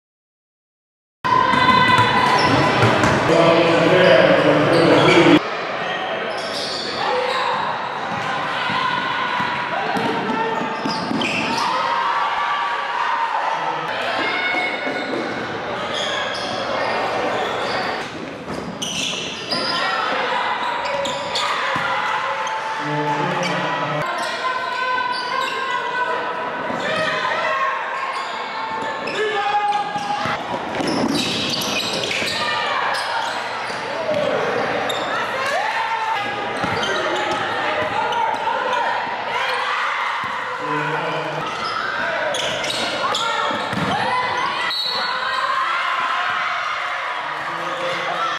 CLASS OF 2022 F AMAYA MOSS | KELL HIGH SCHOOL
5'10 F AMAYA MOSS
CLASS OF 2022 | KELL HS
PLAYER HIGHLIGHTS DURING HER SENIOR SEASON WITH KELL HS.